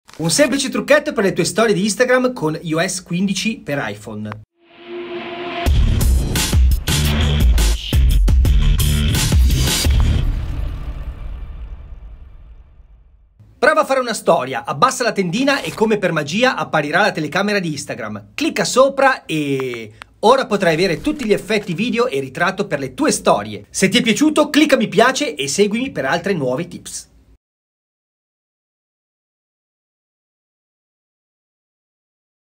I TUOI EFFETTI CON UN SEMPLICE CLICK
Un semplice trucchetto per le tue storie di Instagram👌

Seguimi per altre Tips